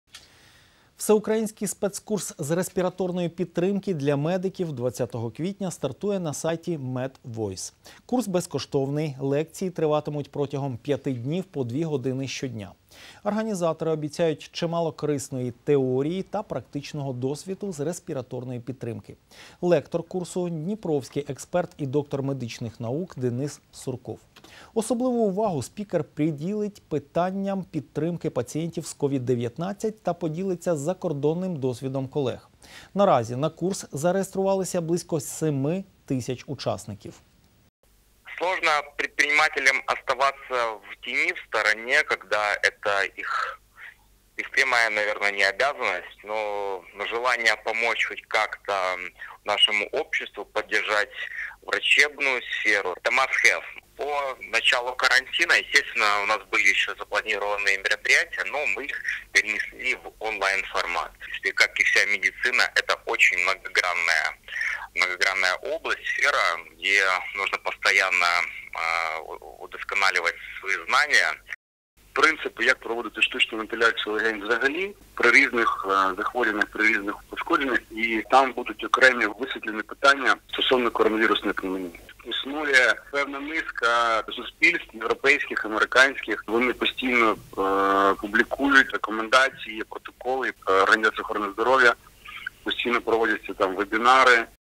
Всеукраїнський спецкурс з респіраторної підтримки для медиків 20 квітня стартує на сайті Medvoice
Курс — безкоштовний, лекції триватимуть протягом 5 днів по 2 години щодня. Організатори обіцяють чимало корисної теорії та практичного досвіду з респіраторної підтримки. Лектор курсу — дніпровський експерт і доктор медичних наук Денис Сурков. Особливу увагу спікер приділить питанням підтримки пацієнтів з COVID-19 та поділиться закордонним досвідом колег.  Наразі на курс зареєструвалися близько 7 тисяч учасників.